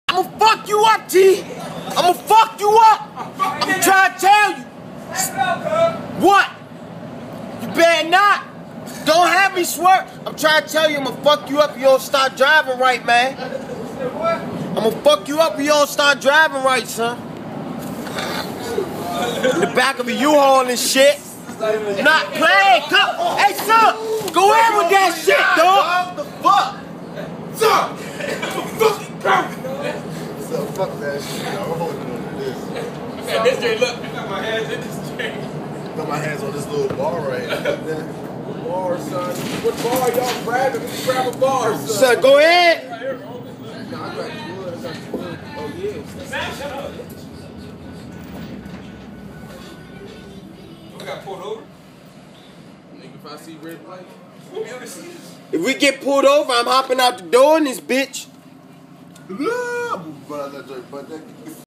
HefnerMob In The Back Off A UHaul " Funny Shit"
In the movin truck an Young Hef Is Swearvin while we in the back, Must Seee!!!